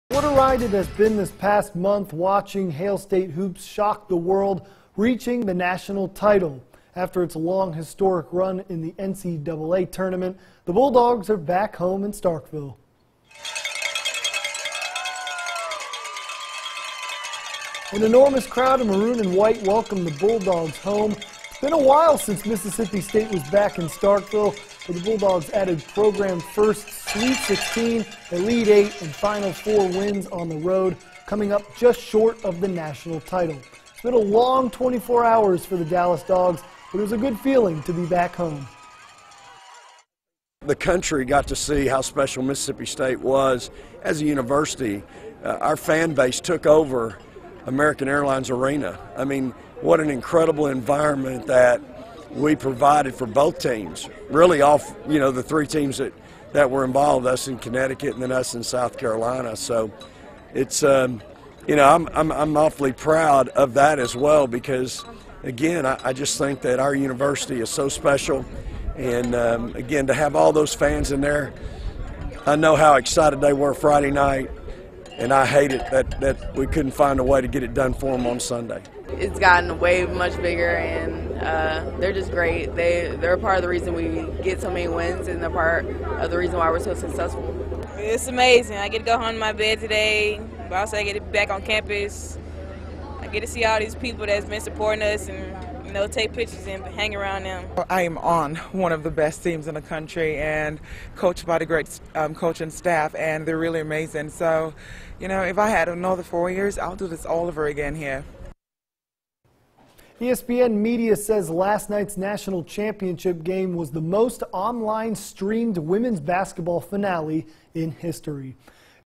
msu welcomed home
Mississippi state welcomed home after title game